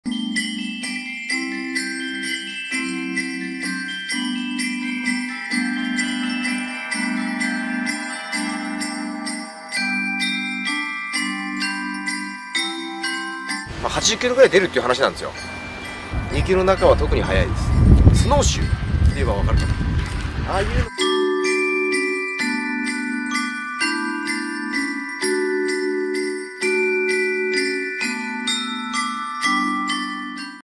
Domino's pizza entrenó renos para hacer domicilios esta navidad
La sucursal de Domino's Pizza de Japón está probando un nuevo método de entrega entrenando renos para llevarle la pizza a los clientes.